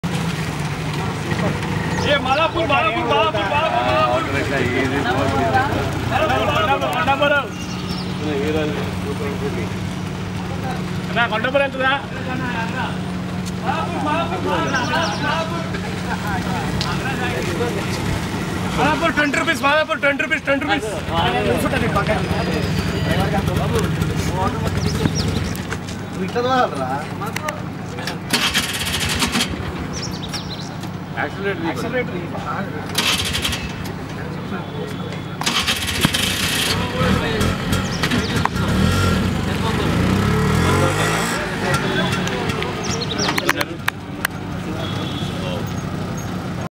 Autoraja Priyadarshi| Women Beware| Comedy| Mithai the Film| Behind the scene
Our fellow Autodriver, he has the charm! | Behind the scene masti |  Produced & Directed by Prashant Kumar|  Music by Vivek Sagar